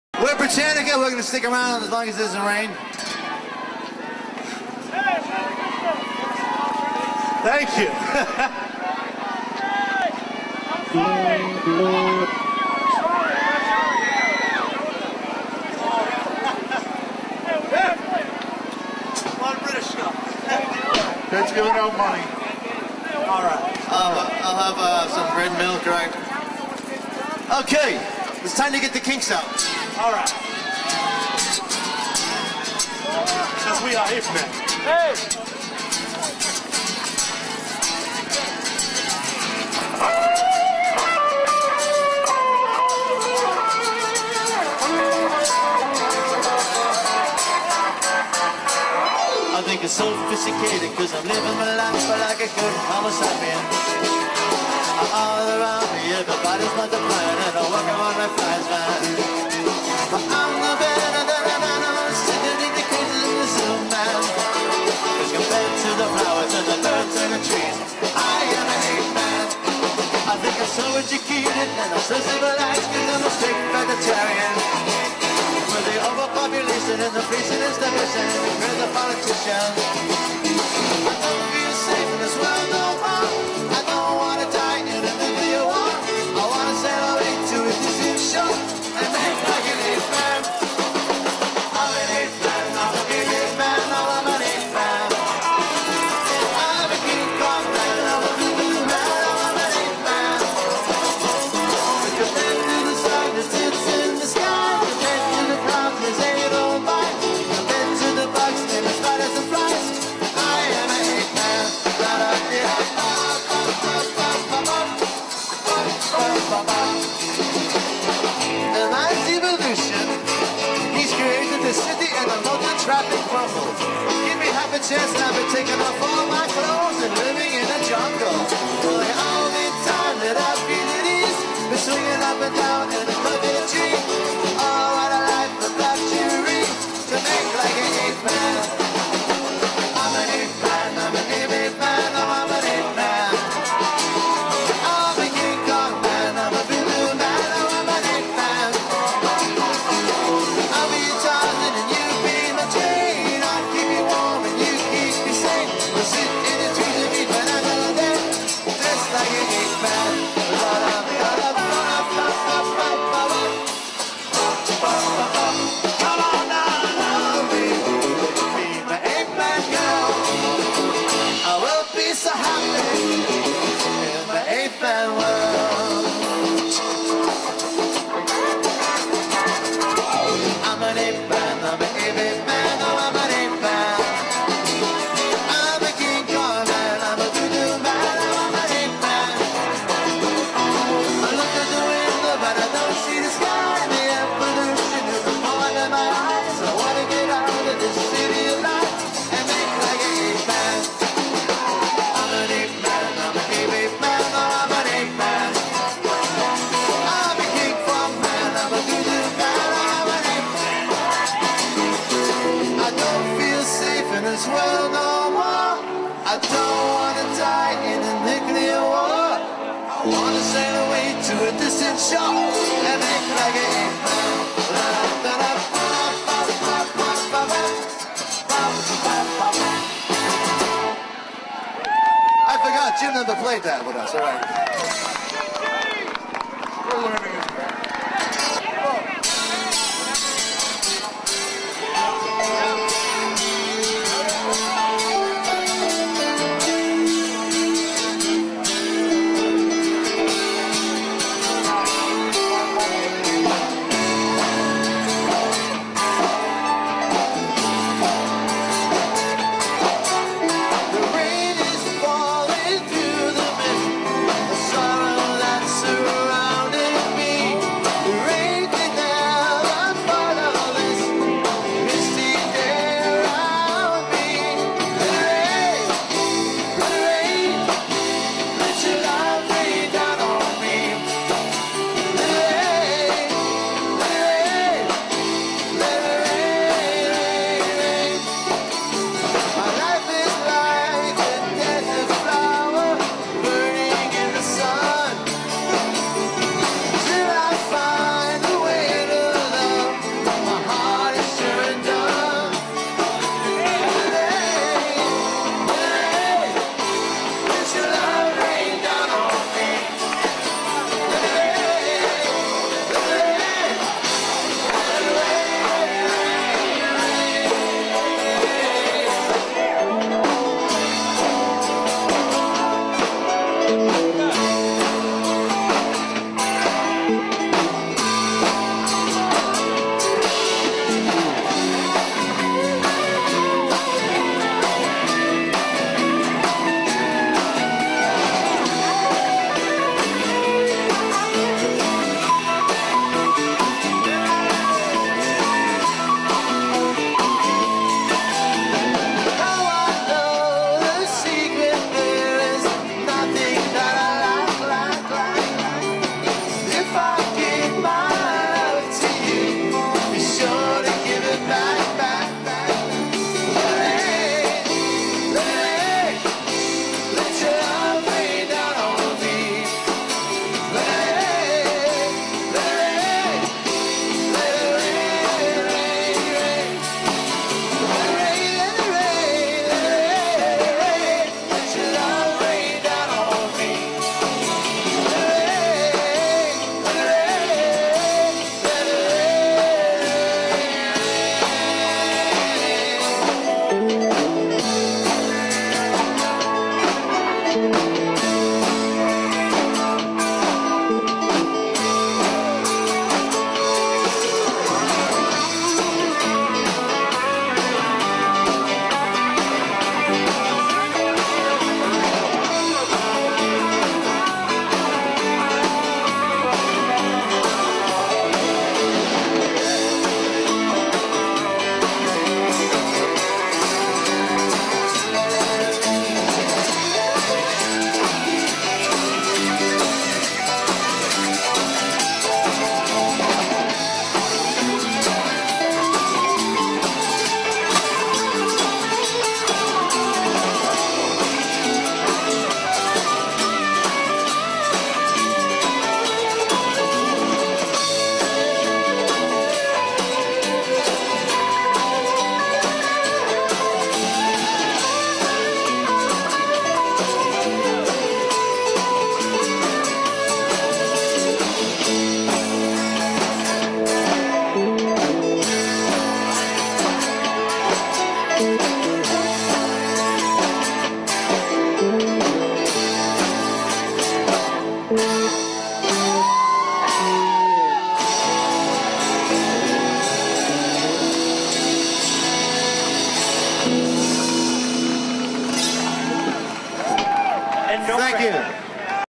britannica halloween night salem ma
A few songs by Britannica Halloween night in Salem, Ma.